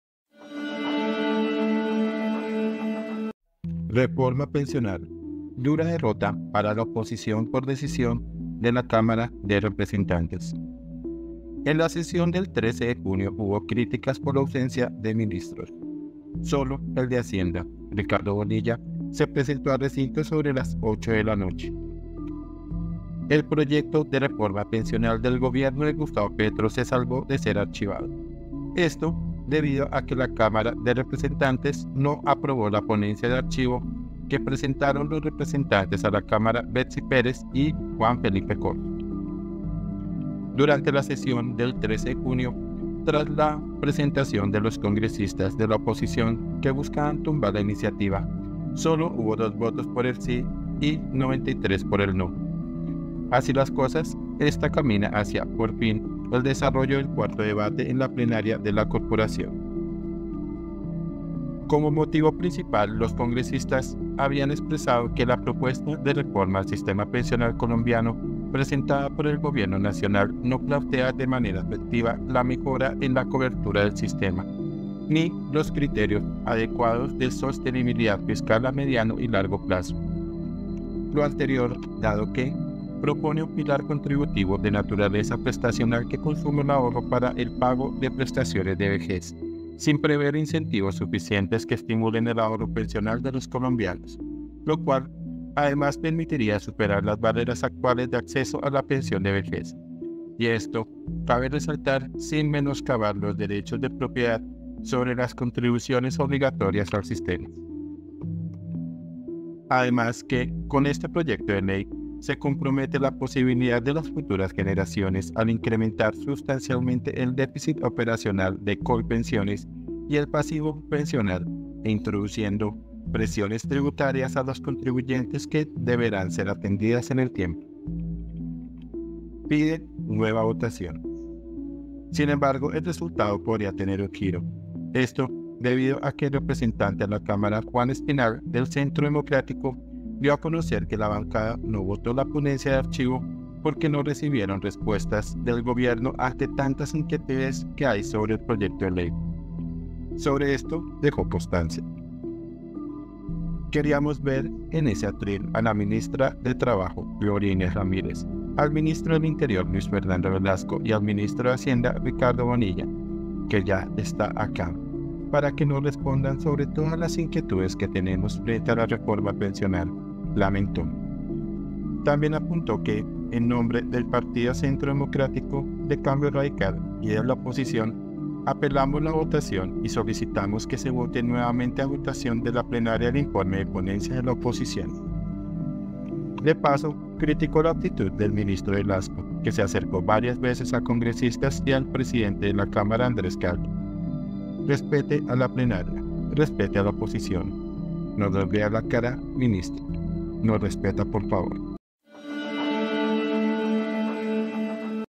⛔ DURA DERROTA PARA LA OPOSICIÓN POR DECISIÓN DE LA CÁMARA DE REPRESENTANTES
Reforma pensional: dura derrota para la oposición por decisión de la Cámara de Representantes
En la sesión del 13 de junio hubo críticas por la ausencia de ministros. Solo el de Hacienda, Ricardo Bonilla, se presentó al recinto sobre las 8:00 p. m.
INFOBAE